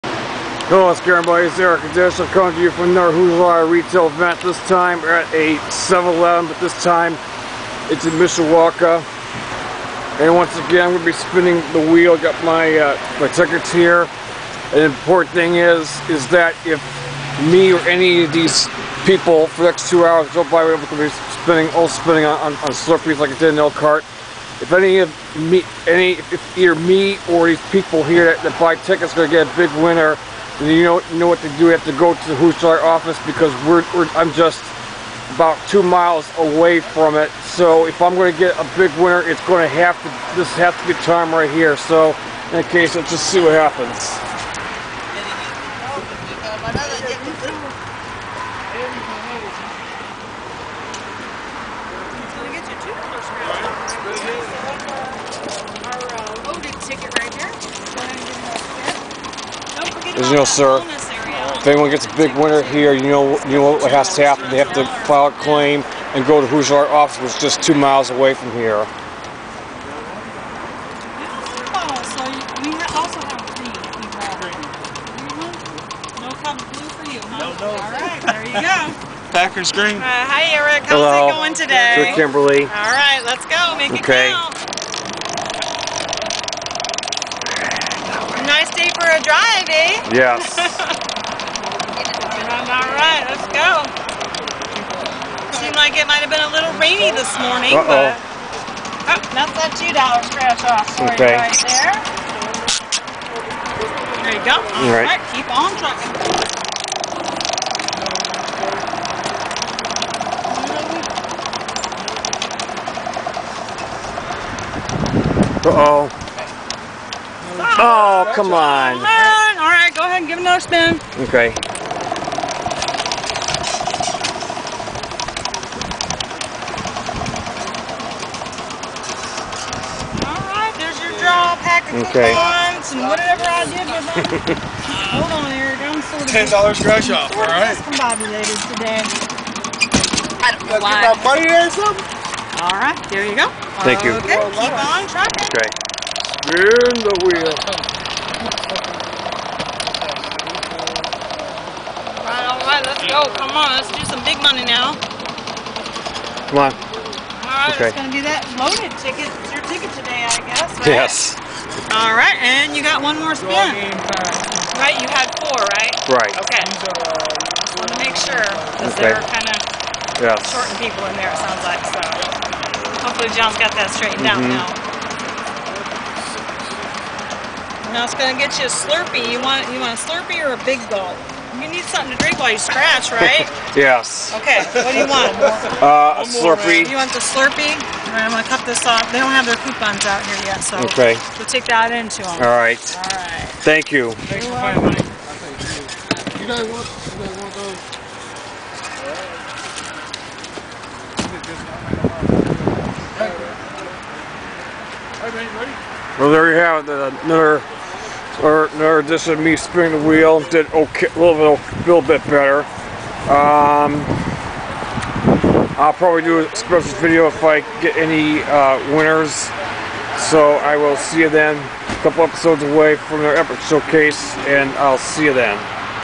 Hoosier Lottery Retail: Mishawaka and 7-11 Again
My road trip continues with another Hoosier Lottery gig as they land in Mishawaka again (two miles from their office!) and at another 7-Eleven (where they had one in Elkhart back in the spring). I again spin the wheel to win some prizes.